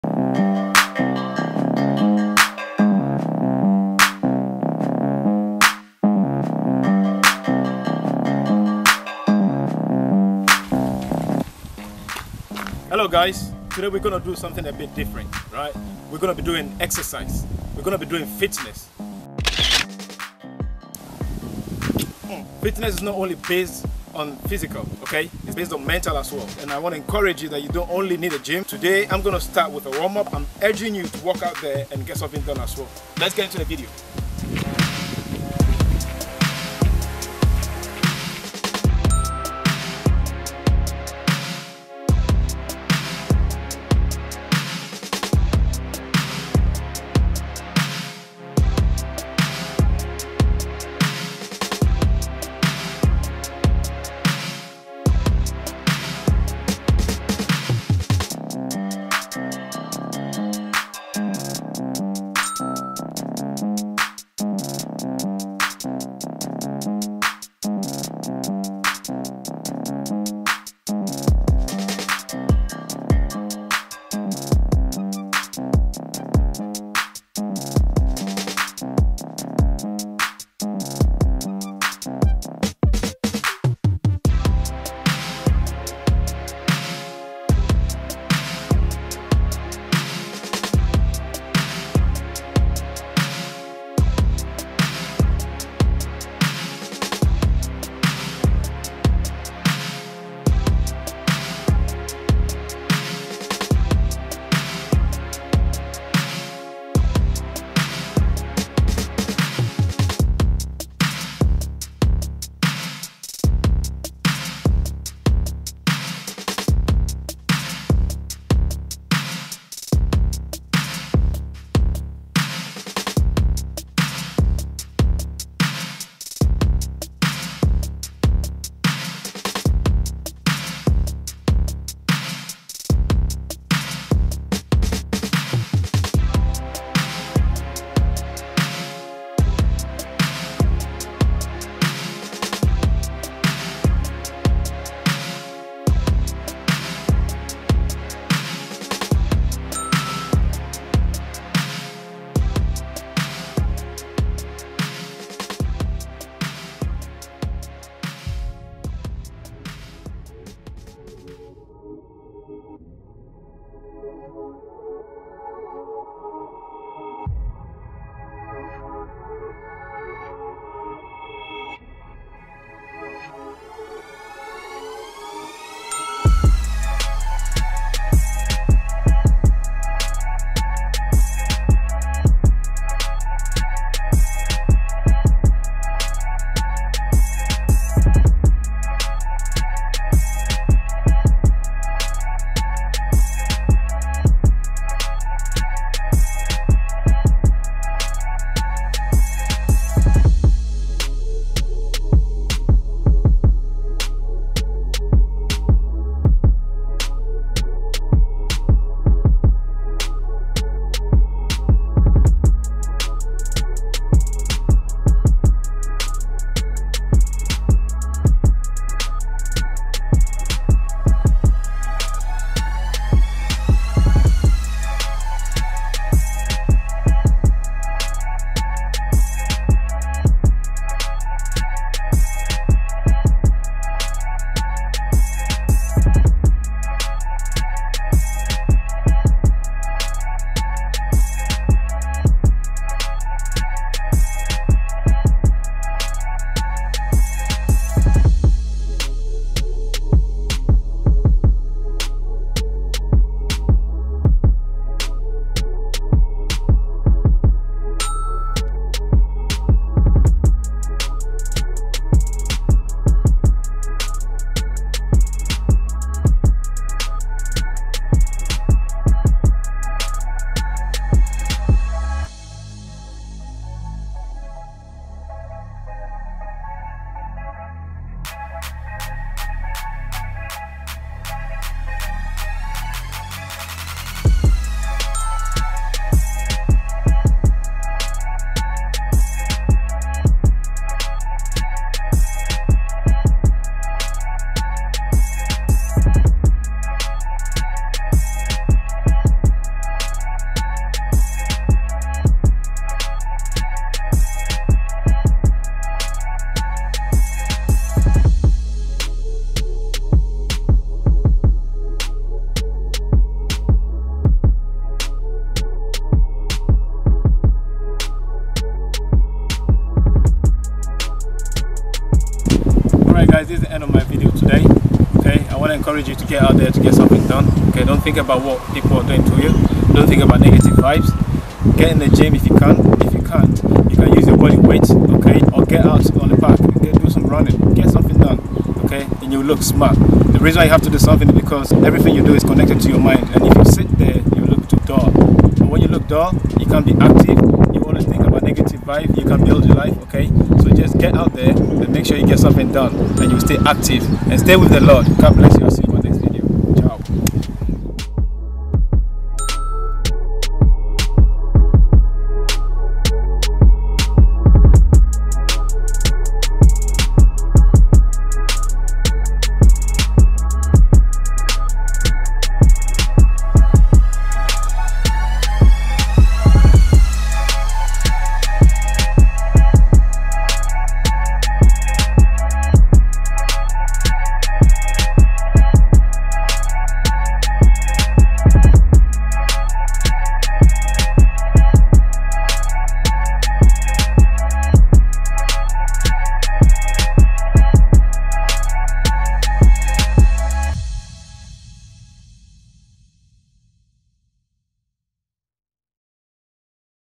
OUTDOOR WORKOUT SESSION | BE MOTIVATED!
Today I  am taking you guys with me outside for some fitness! Remember, to stay motivated and the only person who can stop you is YOU! I hope you enjoy this video.